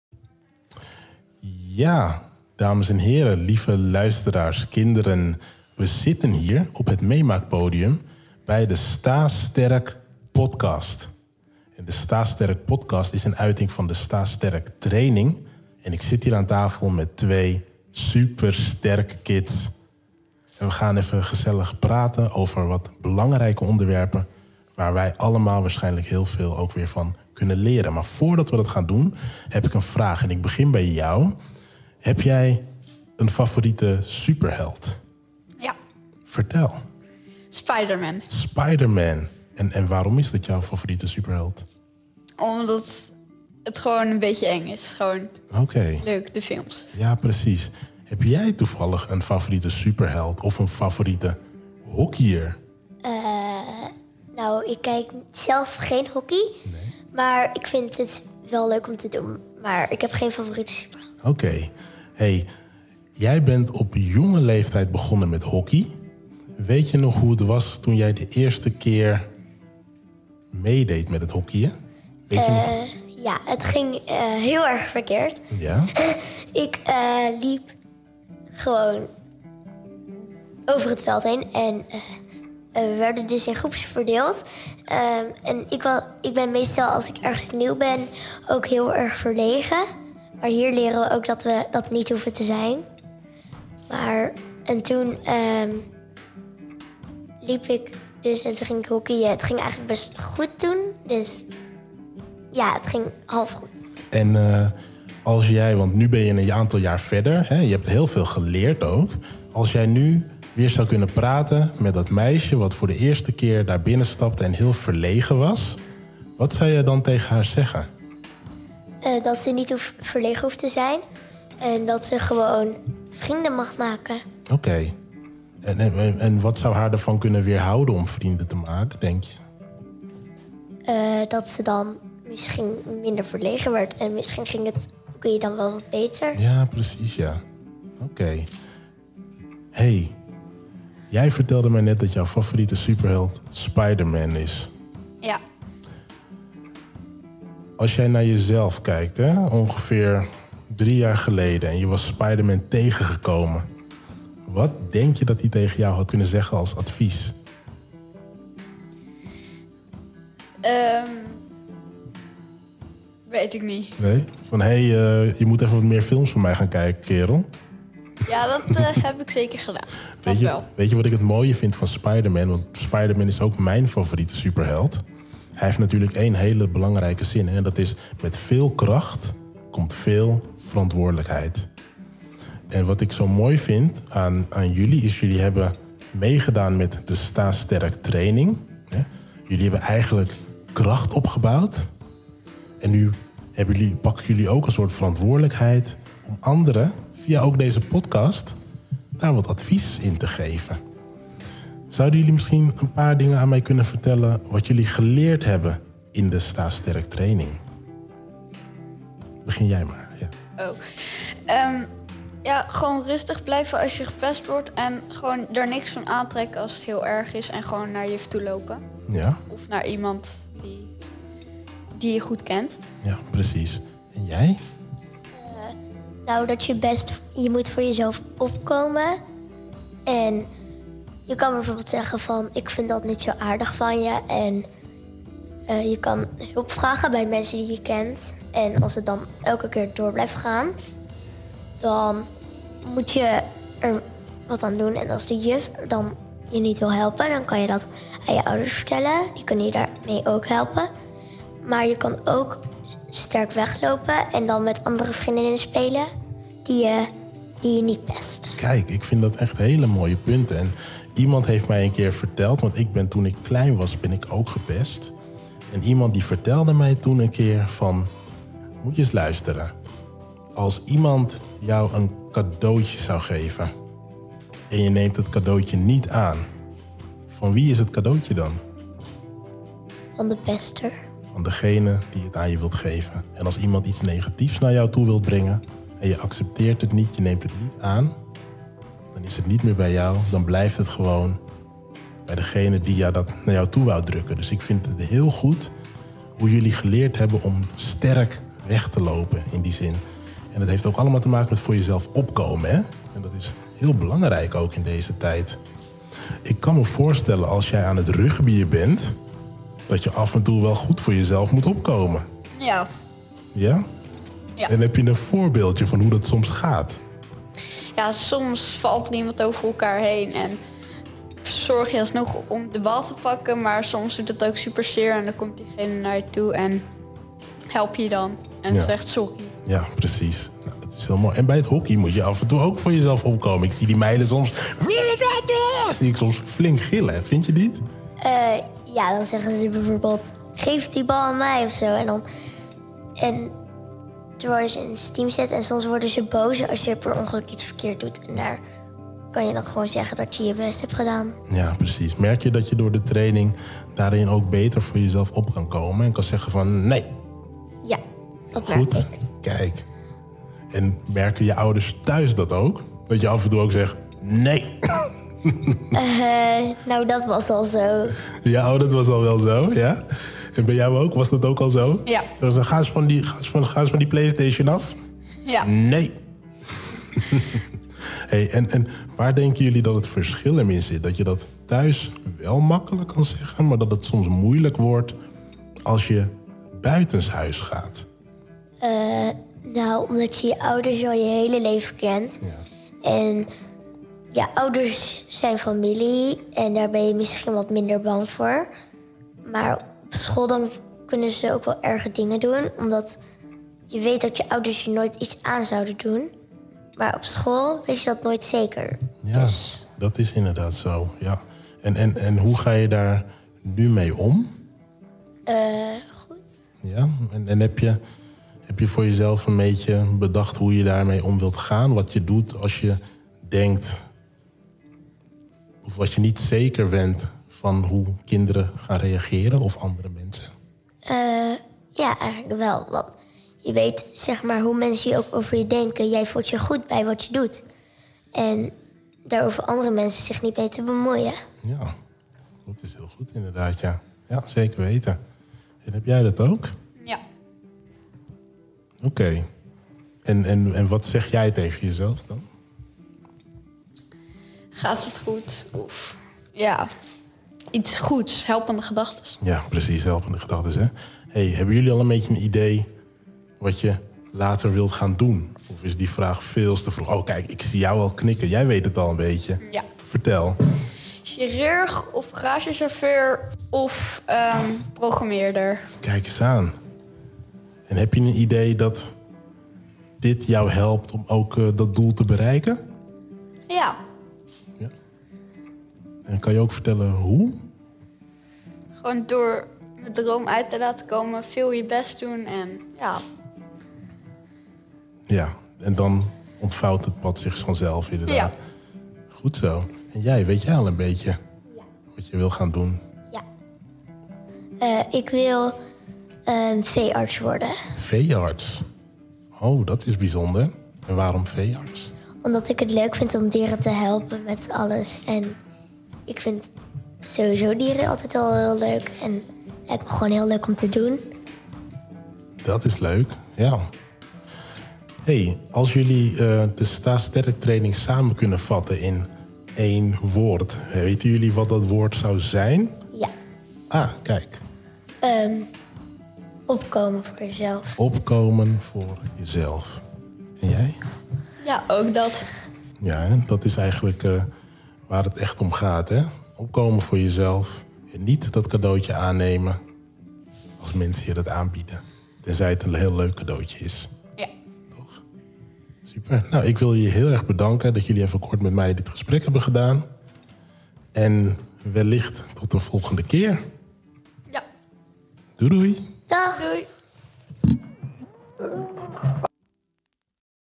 Sta Sterk Podcast | Aflevering 1
Tijdens de Sta Sterktraining ontdekten kinderen spelenderwijs onder meer hoe je voor jezelf kan opkomen, nee kan zeggen, helpende gedachtes kan maken en hulp kan vragen als je het alleen niet kunt oplossen. Als afsluiting werd in samenwerking met de Bibliotheek deze podcast opgenomen. Hier vertellen de kinderen hun ervaringen.